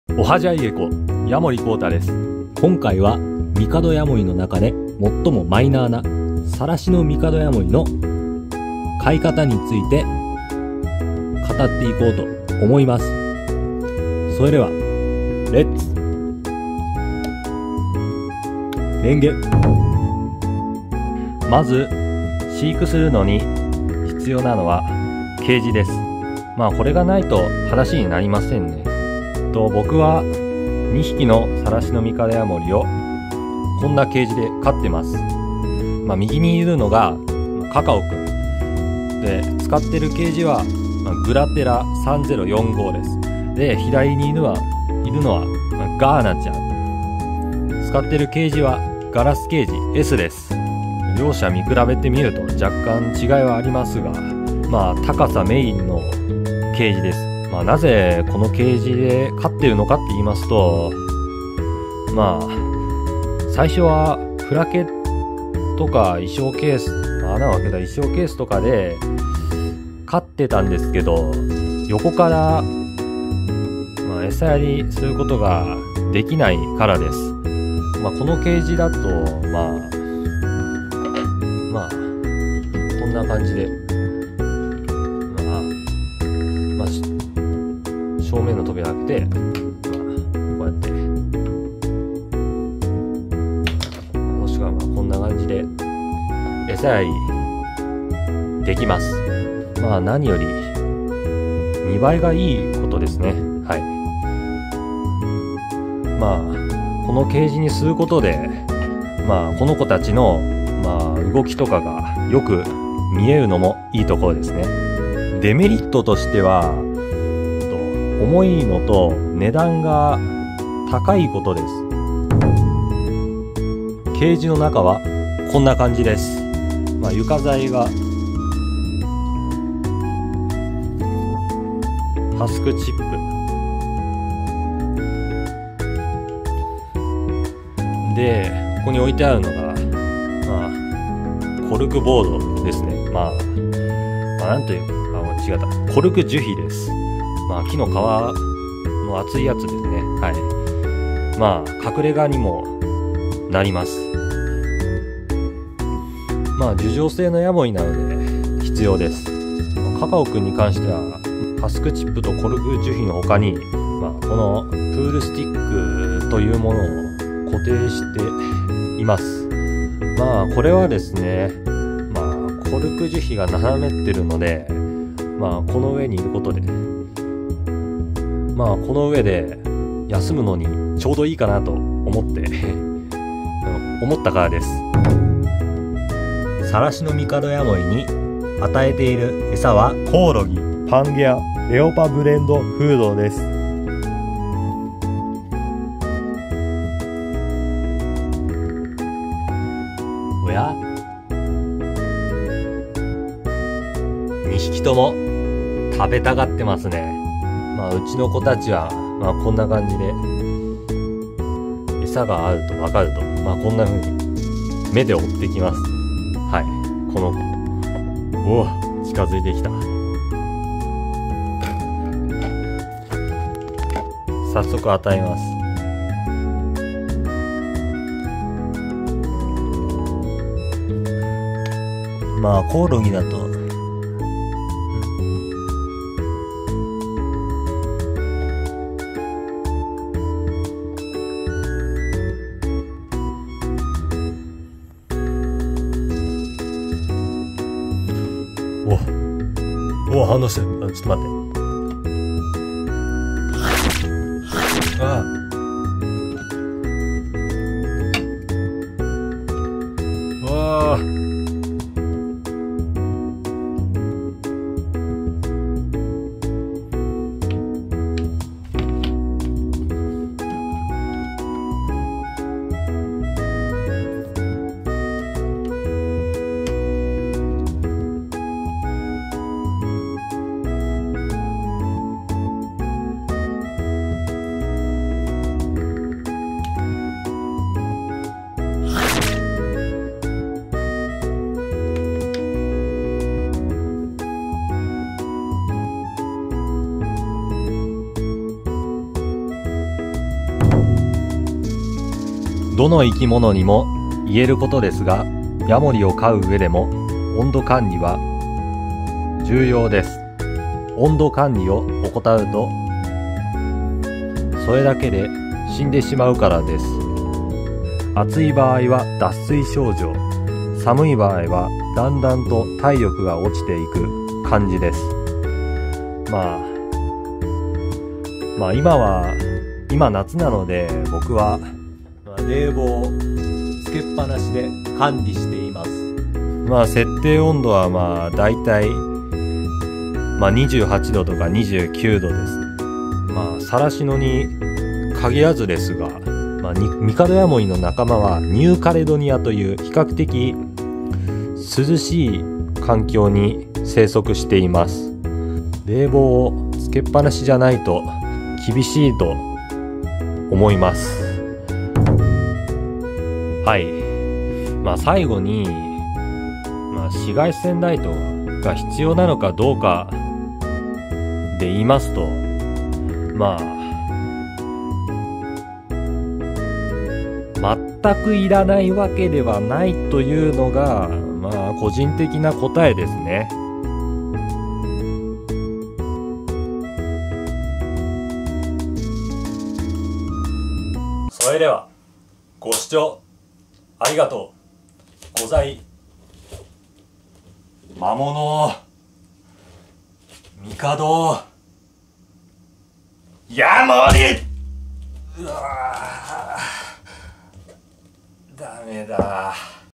マイナーなヤモリの飼育方法【サラシノミカドヤモリ】
おはジャイゲコやもりこうたです！
今回はサラシノミカドヤモリの飼い方について色々語る動画です。勿論、これが正しいと言うのがないので個人的に思ったことの話がほとんどです。これから飼おうと思ってる人はあくまでも参考程度に見てください。